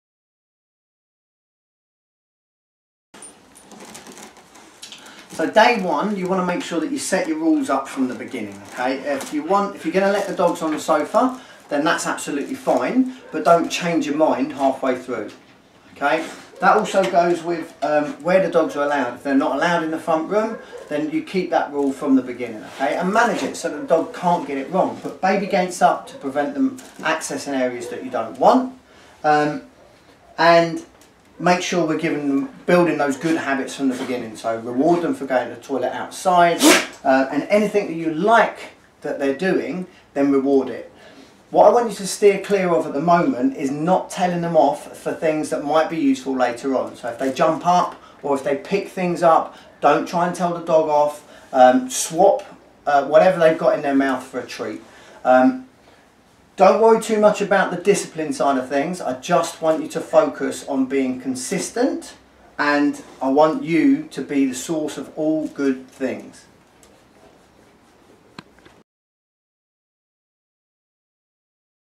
Boundaries for your new rescue dog!
Another video from the care package that was sent out to the contributors of the from Rescue Dog to Super Dog show which would give them some indication about how important it is to be consistent